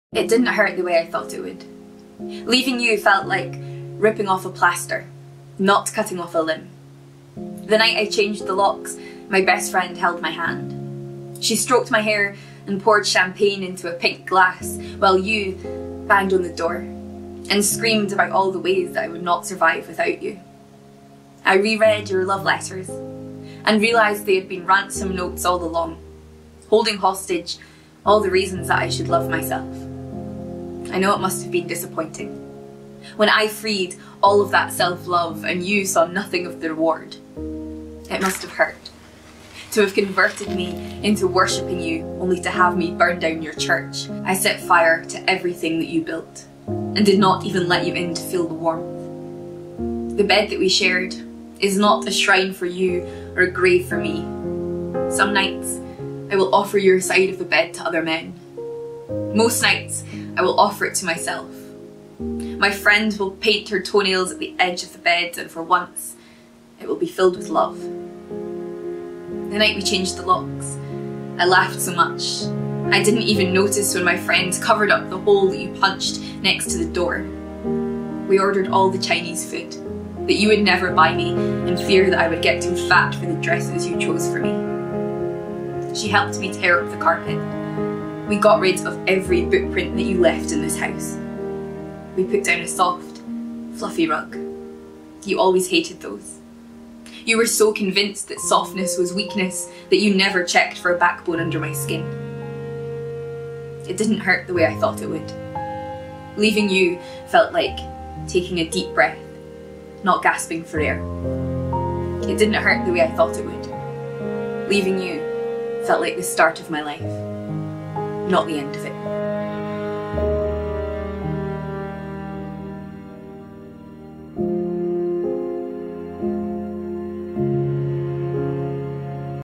Rebuilding | A Poem By Erin May Kelly
Leaving you felt like the start of my life, not the end.

Filmed and edited by Amy Cox.